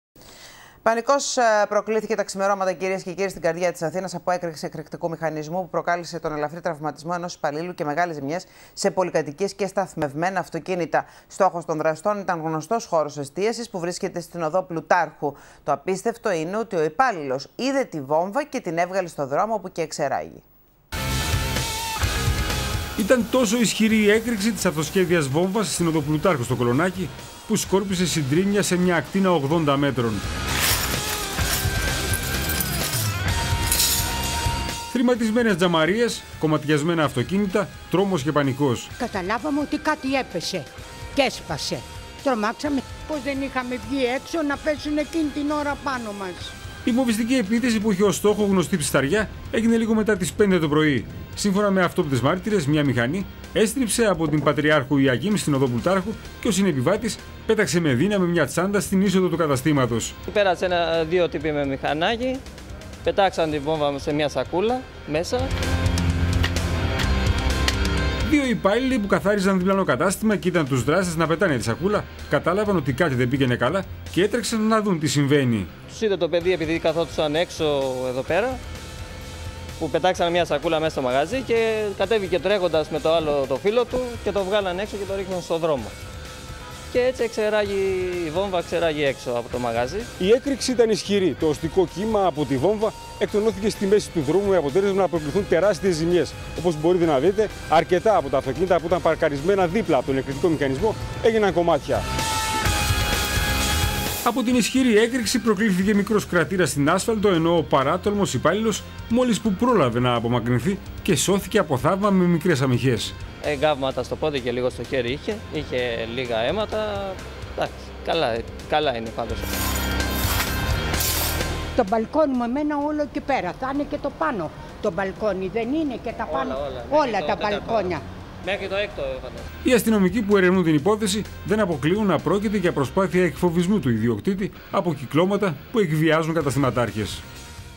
31.7.13 - Πανικός από έκρηξη σε εστιατόριο του κολωνακίου.
Μεσημεριανό δελτίο ειδήσεων, Τετάρτη 31 Ιουλίου 2013, με την Ελένη Τσαγκά.
"Πανικός από έκρηξη σε εστιατόριο του Κολωνακίου."